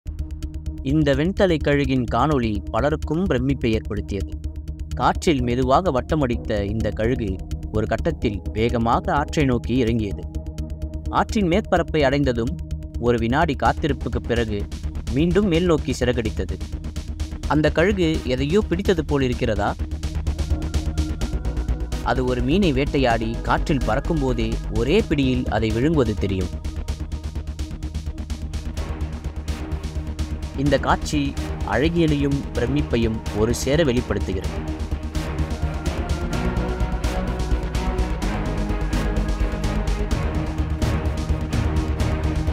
காற்றில் சாகசம் செய்யும் கழுகு - Insane Moment bald eagle catches fish in Mid-air
With astonishing precision, the bald eagle swoops down, swiftly snatching a fish from the water's surface. In mid-air, it devours its prize, showcasing remarkable aerial hunting skills and mastery of flight.